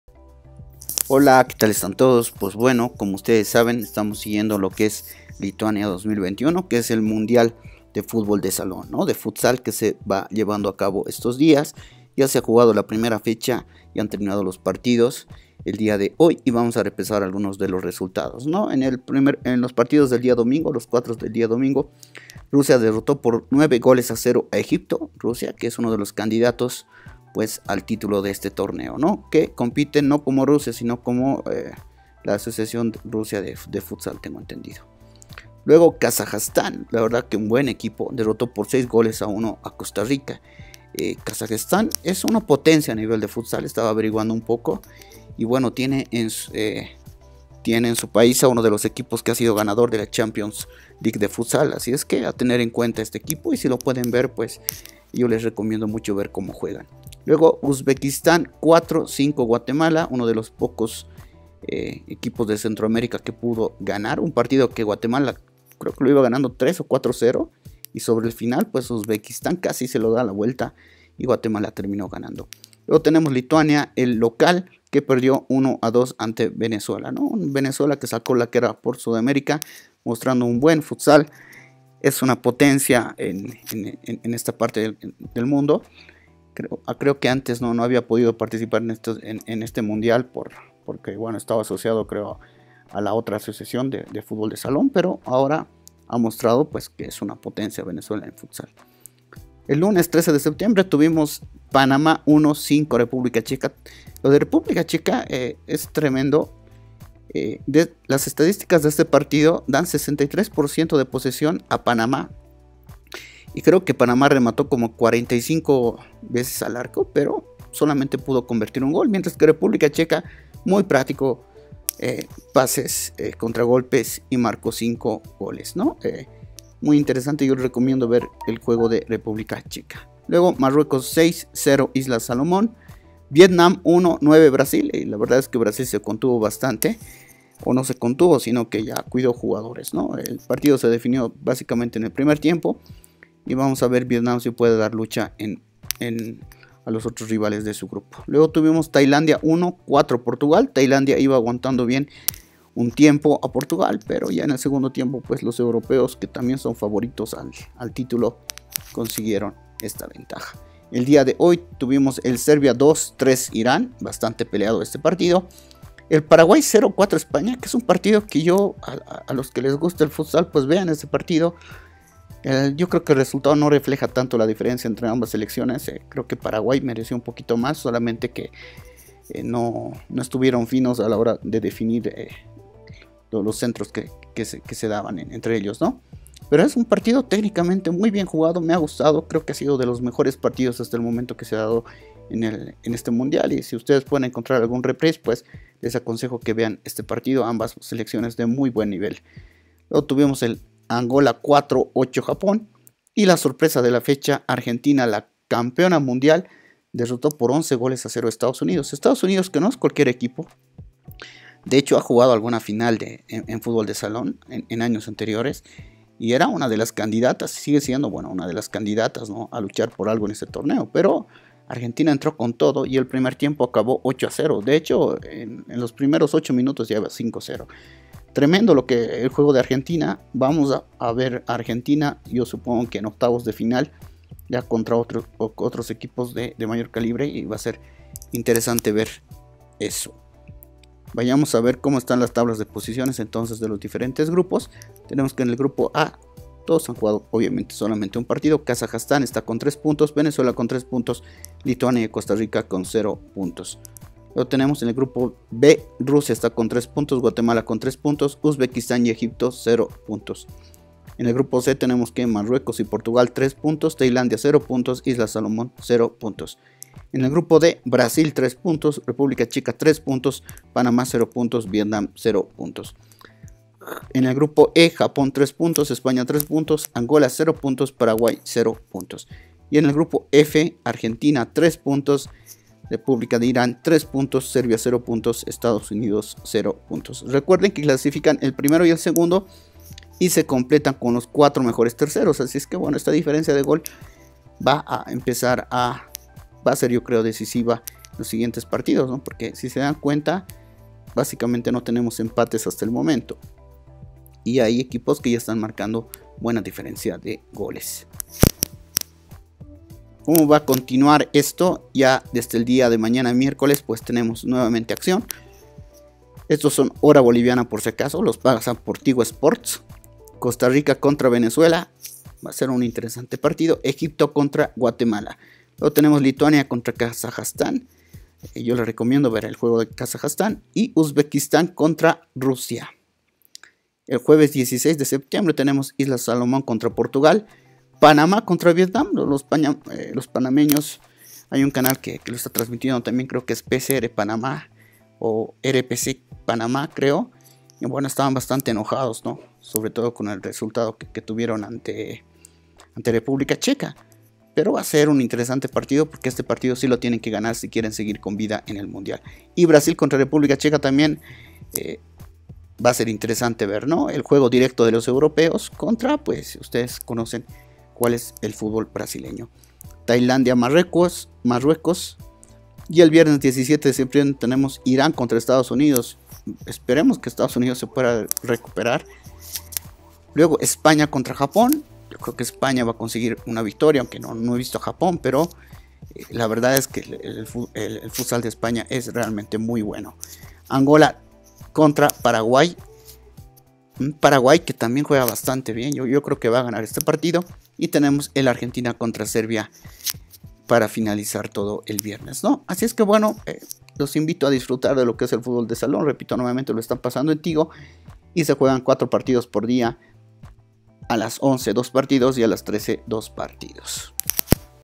Mundial de Futsal Lituania 2021: Resultados Primera Fecha, tabla de Posiciones, Siguiente Fecha
En Este video revisamos la primera fecha del Mundial de Futsal 2021, los resultados, la tabla de posiciones y la siguiente fecha